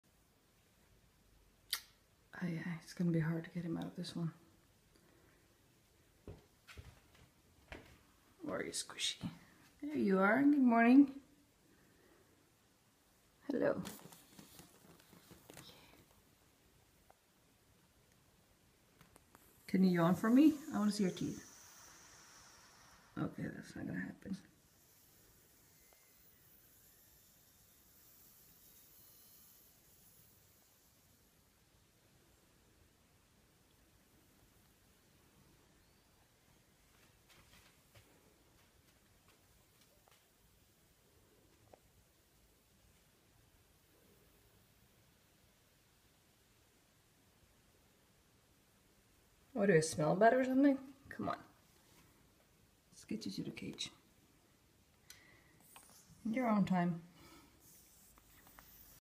My Boa Constrictor
This my Squishy, 15 years old. Only bit me twice 🙂